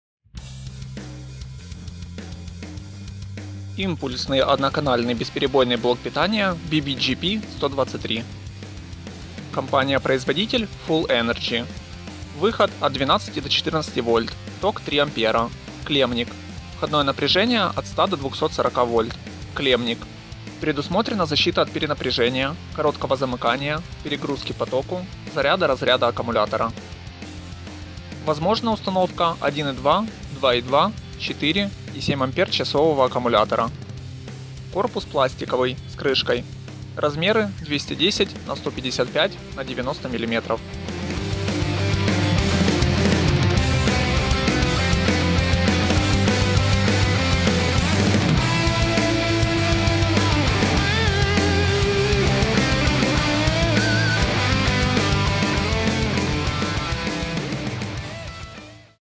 Блок питания Full Energy BBGP-123
Видеообзор - блок бесперебойного питания Full Energy BBGP-123. для систем контроля доступа и охранно-пожарных сигнализаций

Украина, Киев, пр. Бажана, 36